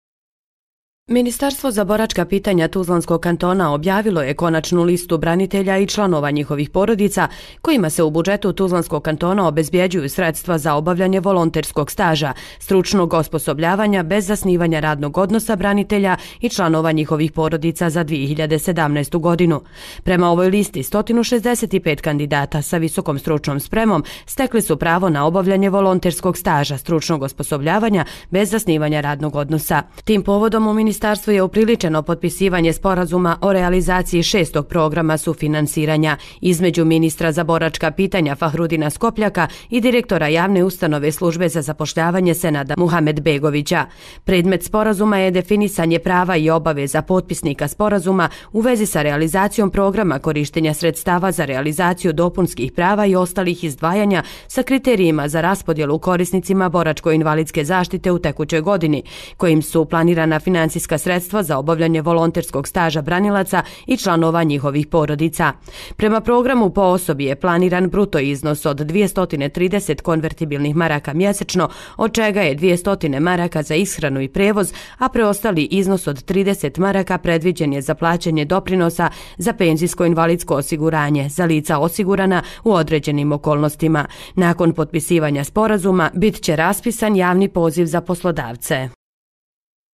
040717 Osposobljavanje boracke
U Ministarstvu za boračka pitanja TK   upriličeno je  potpisivanje Sporazuma o realizaciji VI Programa sufinansiranja obavljanja stručnog osposobljavanja bez zasnivanja radnog odnosa / volonterskog staža boraca i članova njihovih porodica.